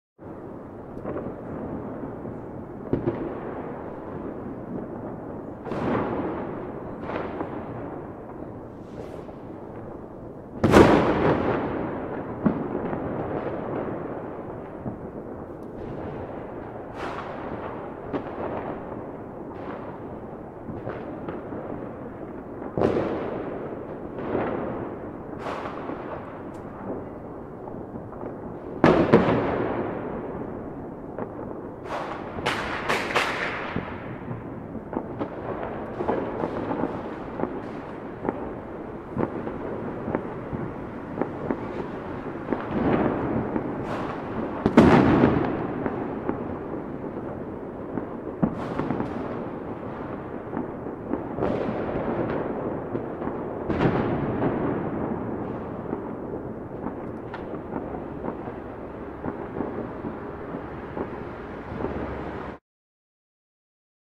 New Year Firecracker Sound Effect 2 | Free Sound Clips | City Sounds
Fireworks at New Year with firecrackers. Rockets. High Quality Sounds. Free mp3 Downloads.